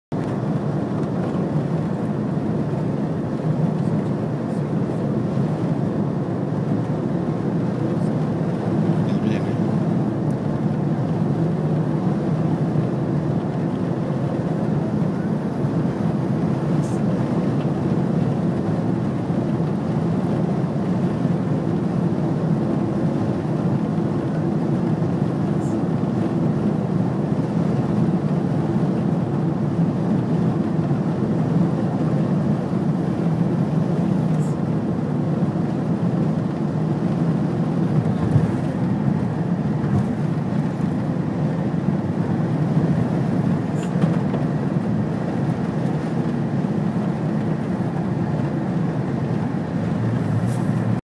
Entrando al estado de Utah
Regresando de yellostone